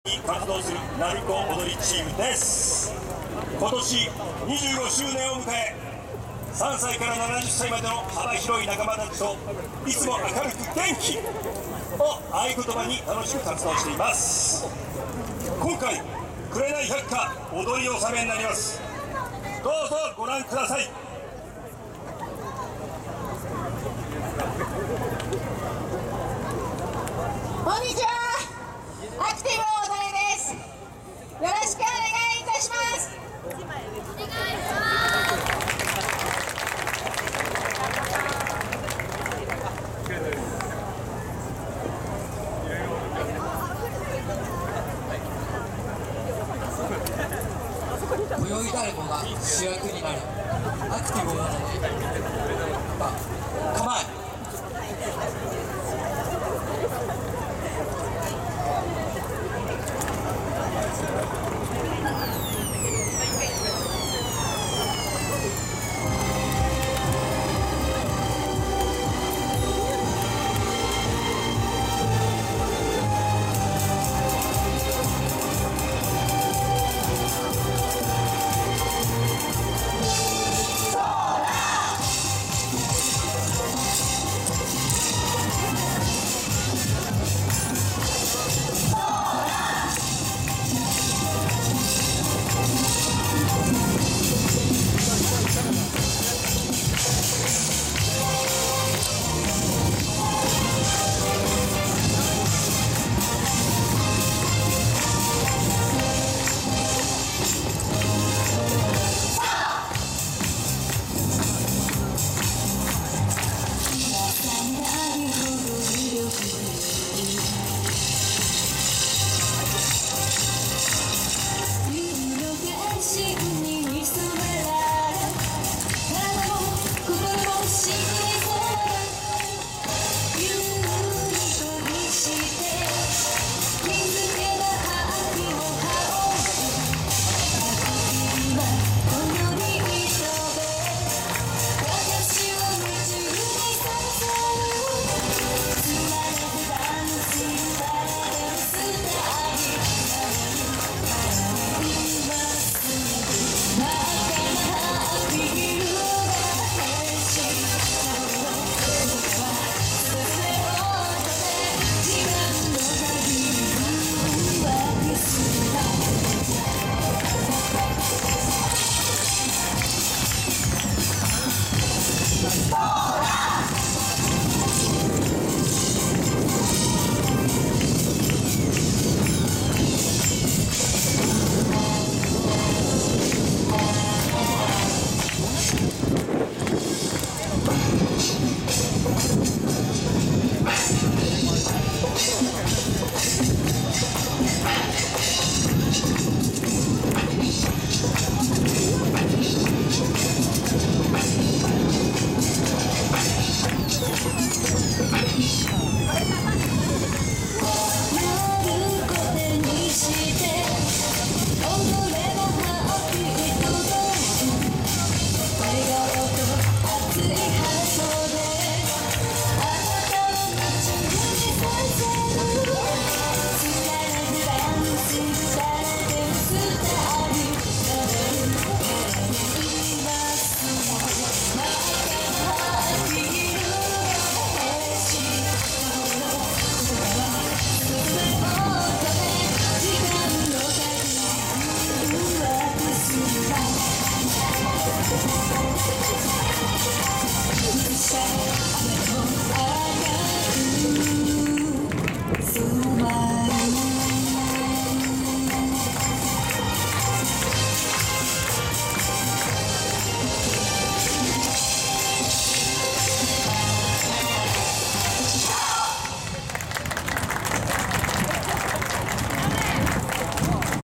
アクティブ大曽根 おかげさま祭り 2023 7 2 1部 アップ
おかげさま祭り　 にて 　アクティブ大曽根　１部　の演舞を撮影させていただきました。